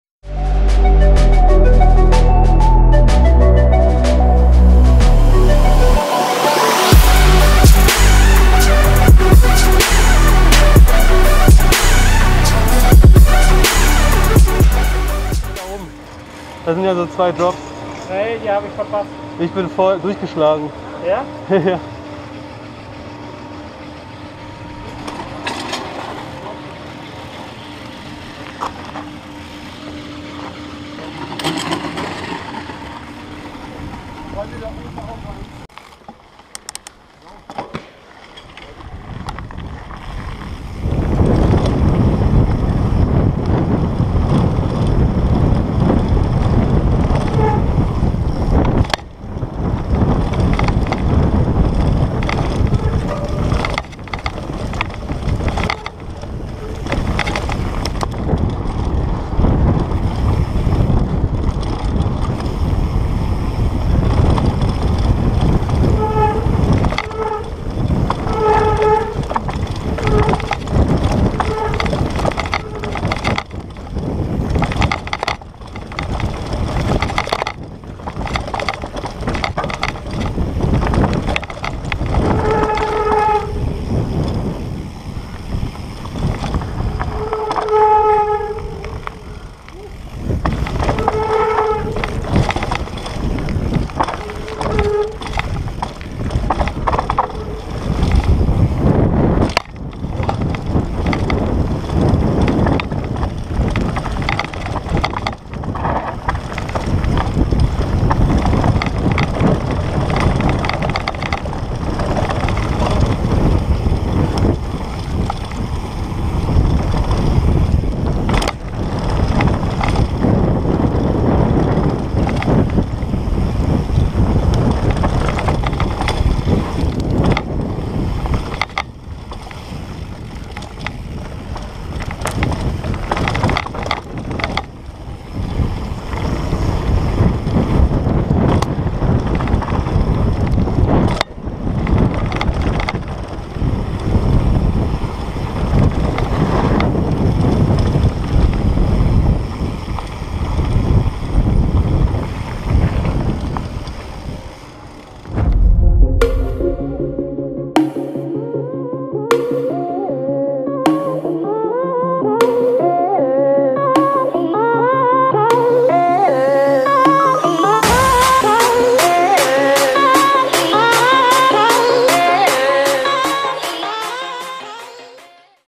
Downhill Line in Bikepark OSTERNOHE - AM Rider - SPECIALIZED STATUS 160 21‘
Really rocky Downhill track.

What I use:
🚲Bike           - Specialized Status 160 2021
⛑Helmet       - Fox Proframe 2020
🥽Goggles   - 100% Accuri
📷Camera    - GoPro Hero 3+ BE